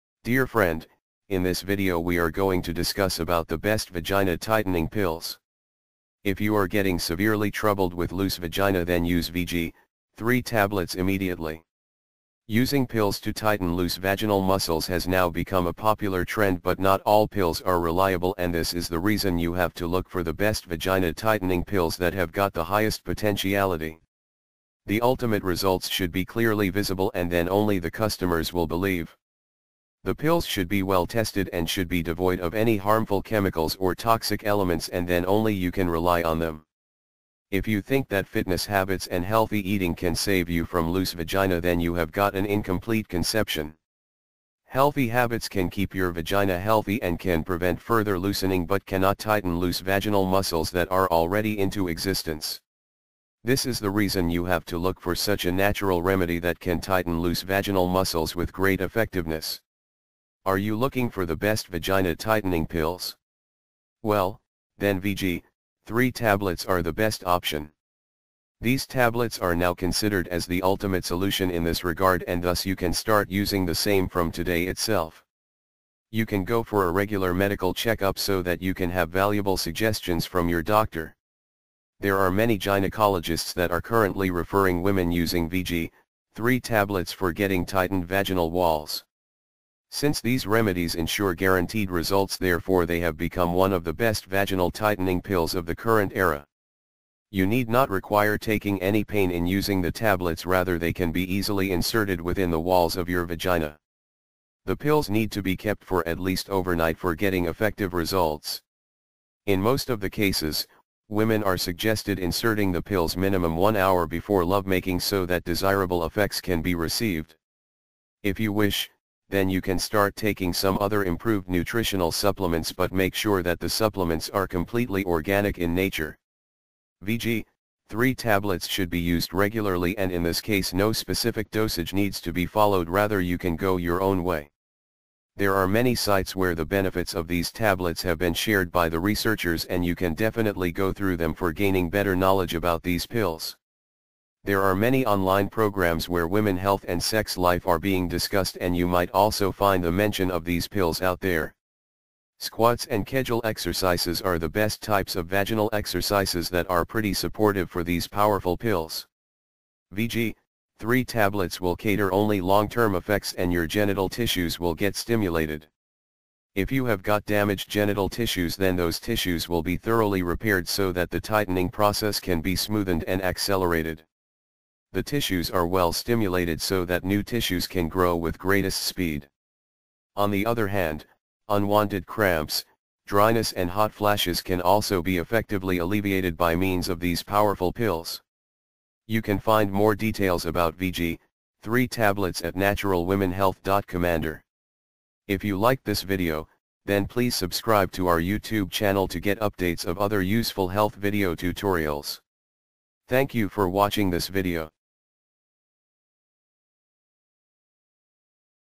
Best Vagina Tightening Pills To Tighten Loose Vaginal Muscles
Dear friend, in this video we are going to discuss about the best vagina tightening pills. If you are getting severely troubled with loose vagina then use Vg-3 tablets immediately.

Best Vagina Tightening Pills